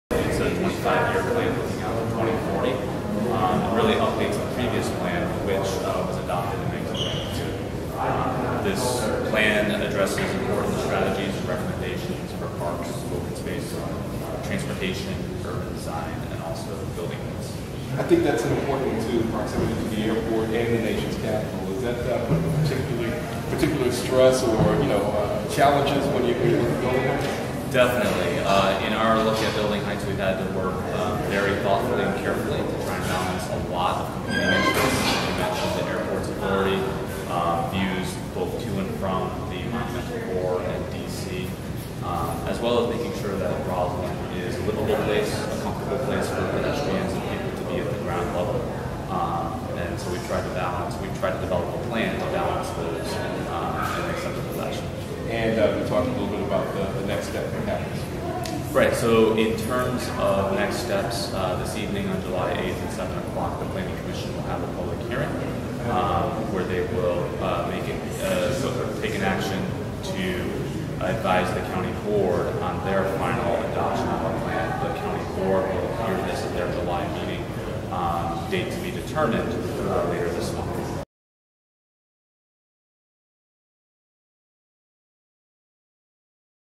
Realize Rosslyn | Anthony Fusarelli Interview | 2015
Arlington County Principal Planner Anthony Fusarelli is interviewed by NBC Channel 4 at the Realize Rosslyn Community Open House.